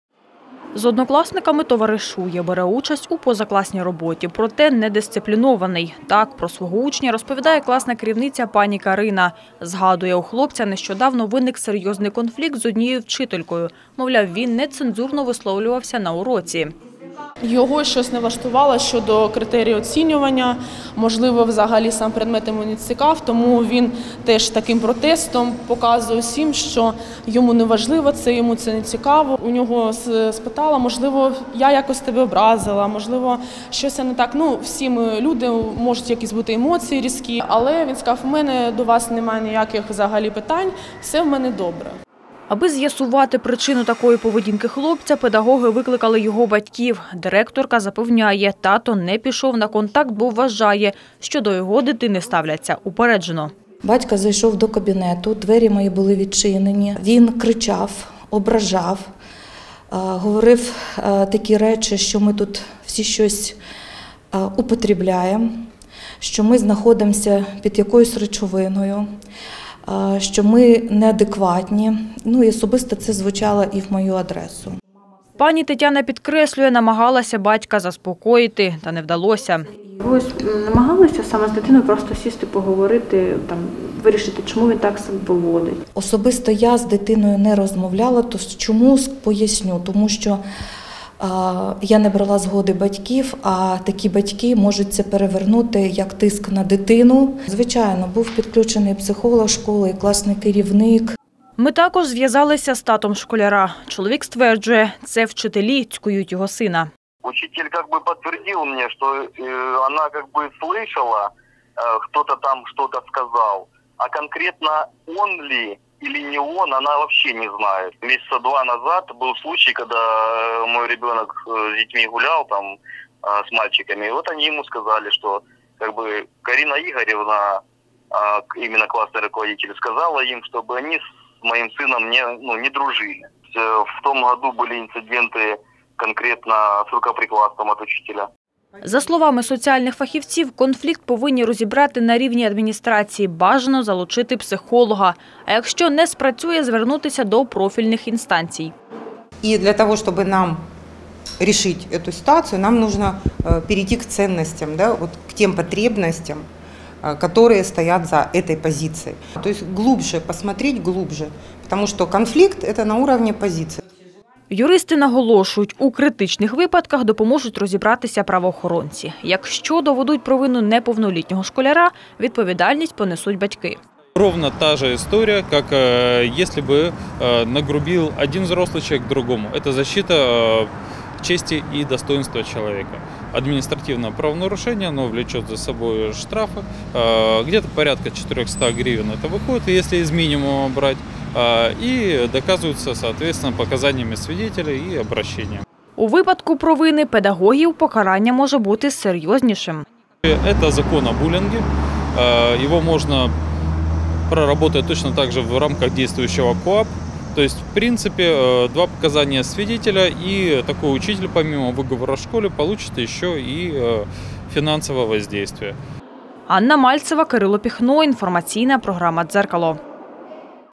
В 63 школе не могут найти общий язык учителя и родители одного из учеников
Грубости, матерщина и пренебрежение. Администрация учебного заведения уверяет: школьник оскорбляет педагогов, зато папа отмечает, что педагоги совершают буллинг в отношении сына.